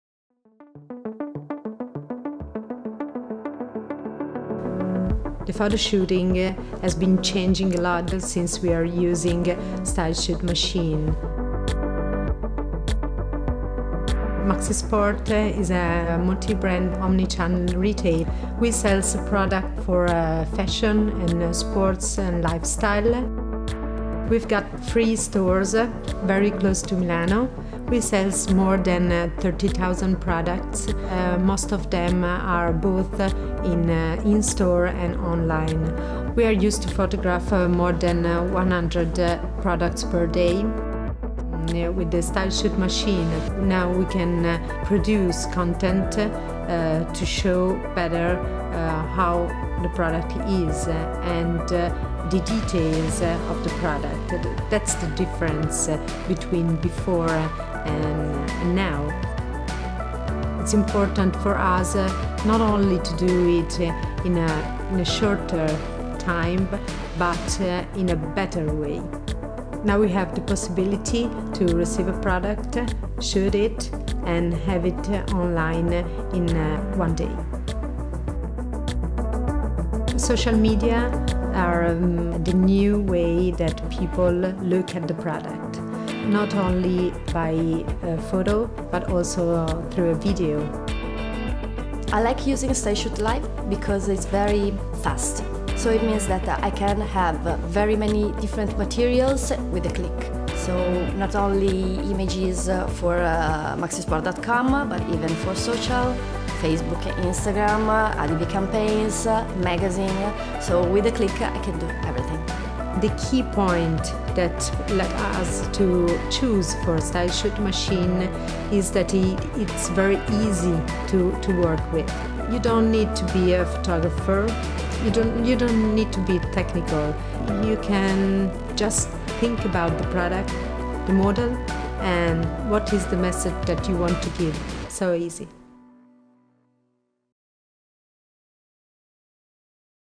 Maxi Sport: Video content made easy at Italy’s premier sports retailer
Using StyleShoots Live, Maxi Sport is now able to receive and publish a new product online in a single day. Maxi Sport is also able to keep the lighting and color consistent between videos and stills by being shot on the same set - something that's almost impossible to do without a lot of color correction.